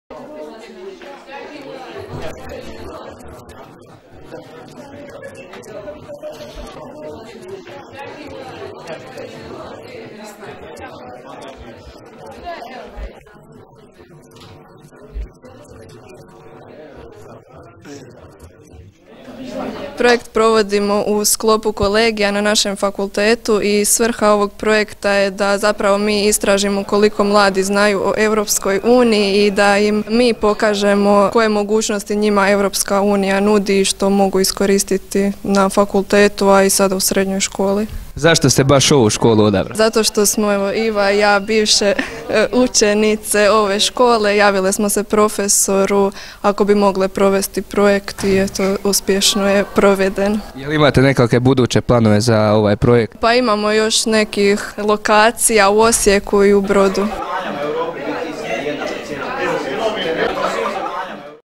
Predavanje o EU [11.05.2017.]
Bivše učenice naše škole Petra Lovrić i Iva Stojčević održale su predavanje o poznavanju Europske unije za učenike trećih i četvrtih razreda.